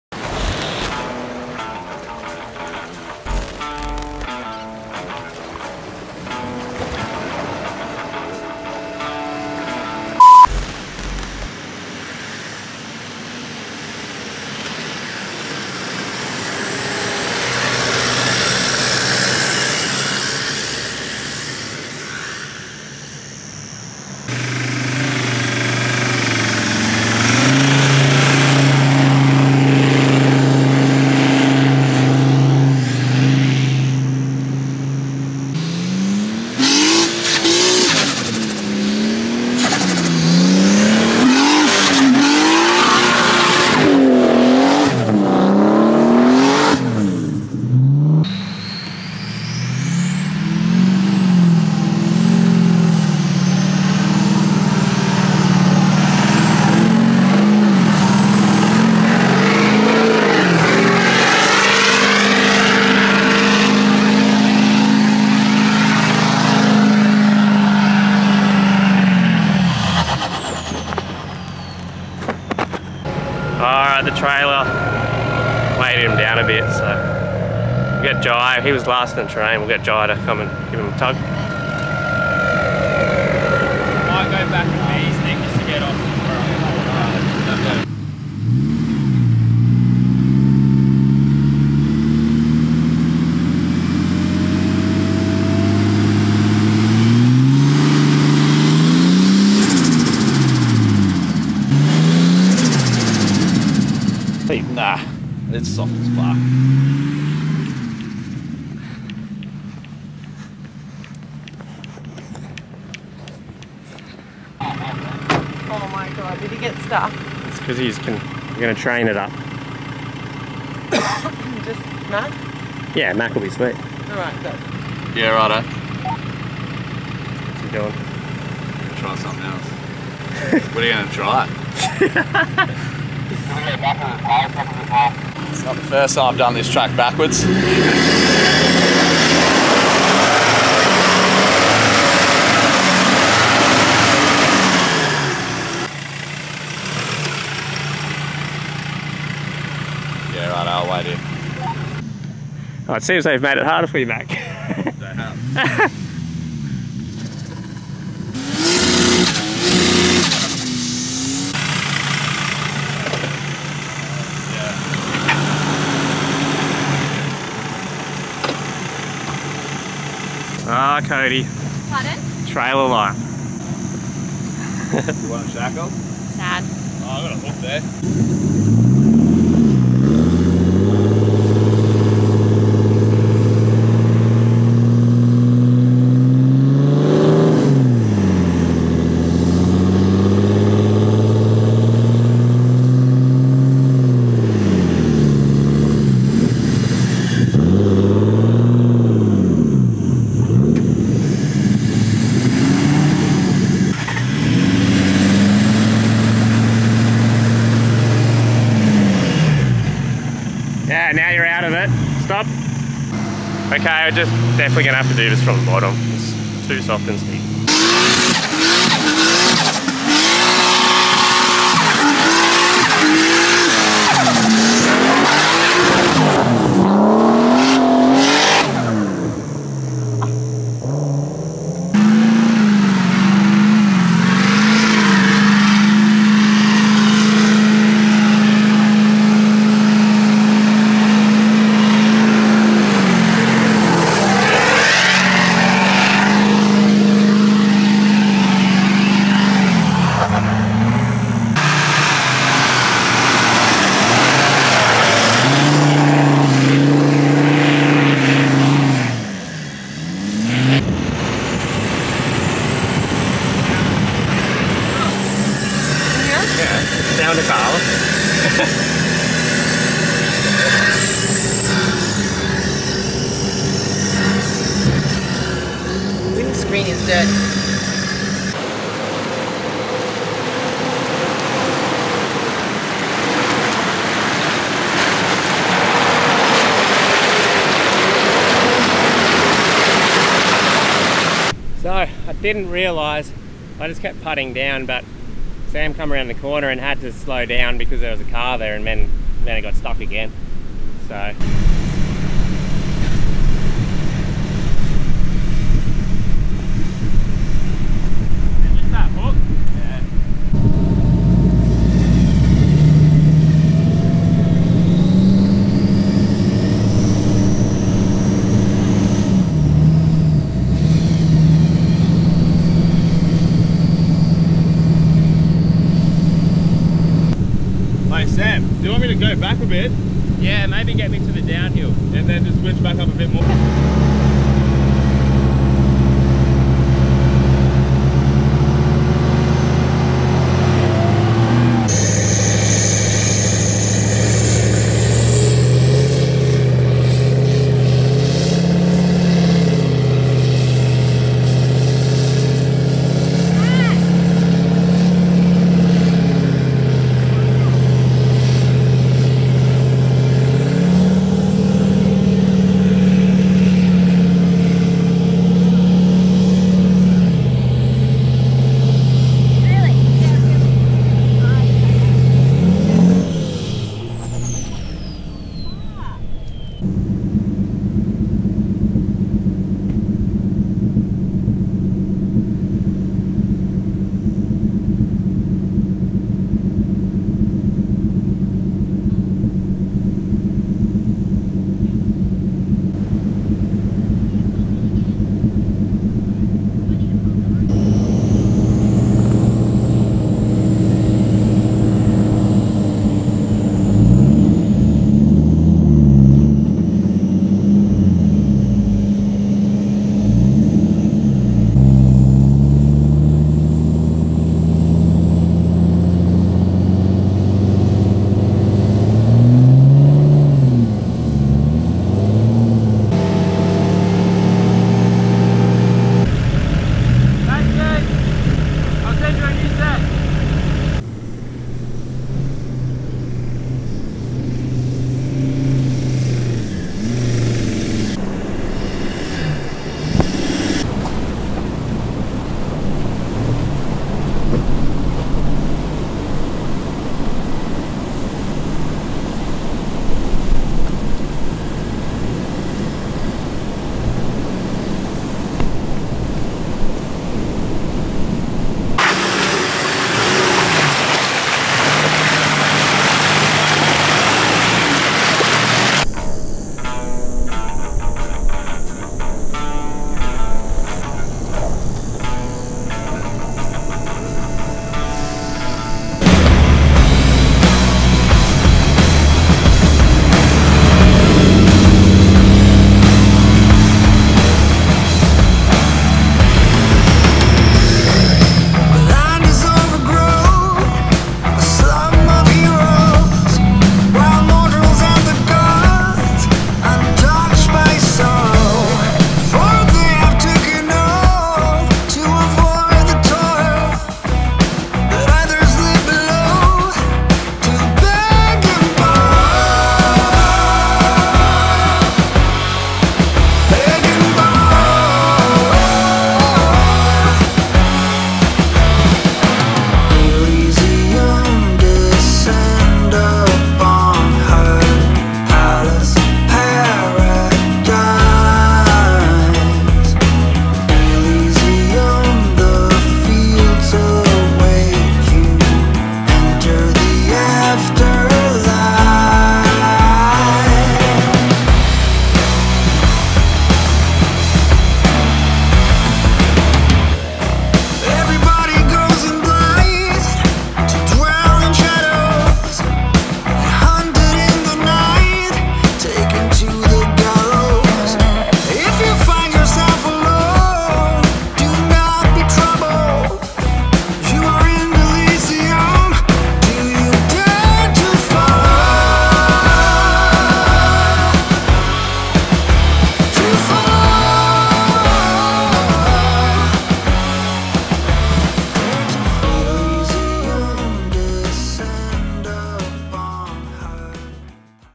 It All went wrong ! 😬
Settle in for a recovery session on the Ngkala track ! bit of different style episode this time !

Find more on INSTAGRAM !

Interested in a Tray & Canopy setup like the Navara? make sure you let them know TJ sent you !